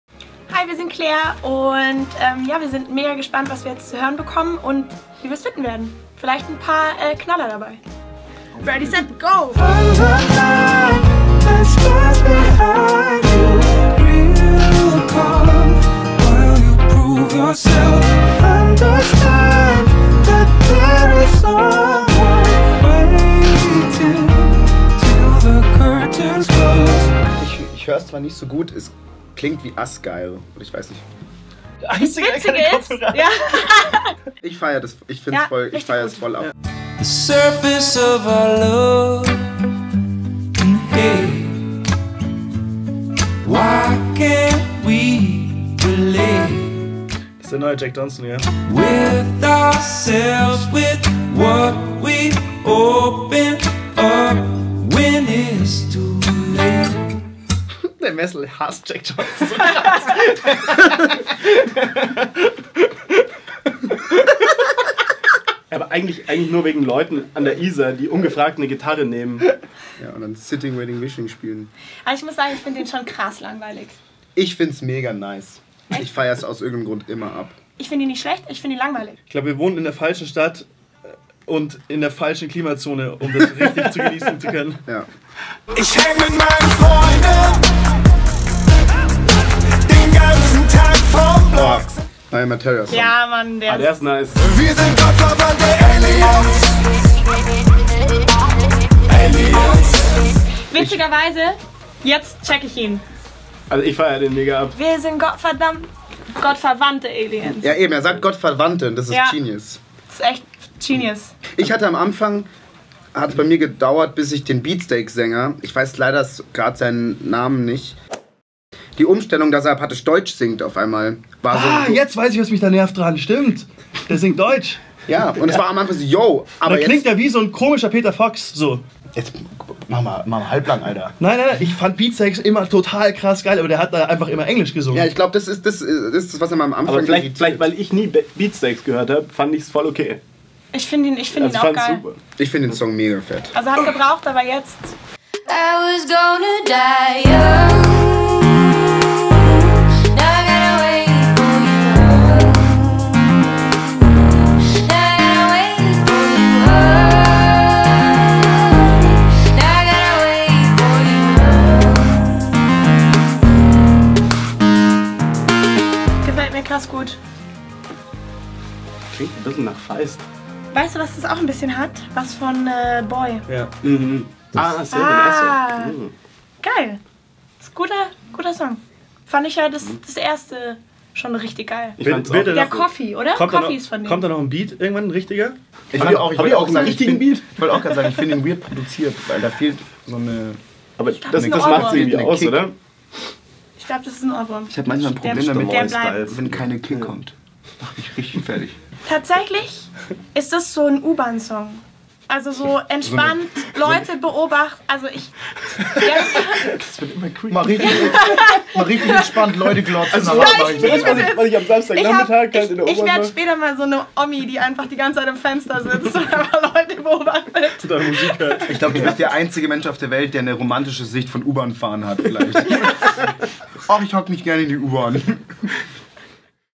Euer erster Eindruck – mit Claire
Josie von Claire fährt also gerne U-Bahn und begafft dabei Leute... jetzt möchten wir nur noch wissen zu welcher Musik. In diesem Video erfahrt ihr es.